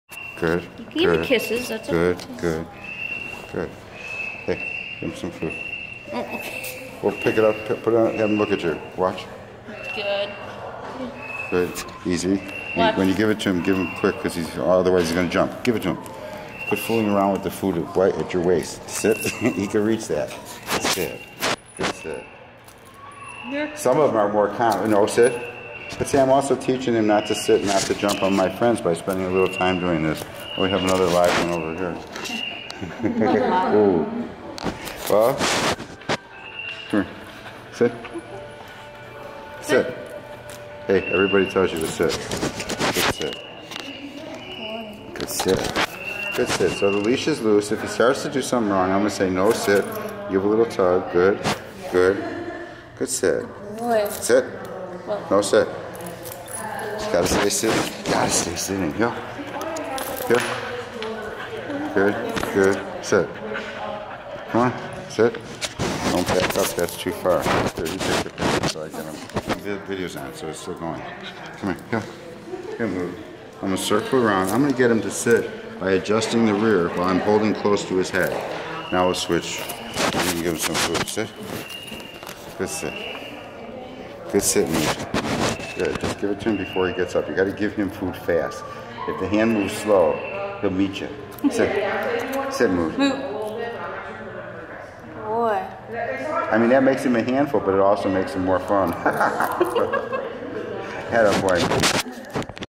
Allan Ross Dog Training
Moot Gibson C. is an 11 month old Soft Coated Wheaten Terrier in his second week of Board and Train.  Here we practice obedience and Meet and Greet without jumping on the staff in the lobby.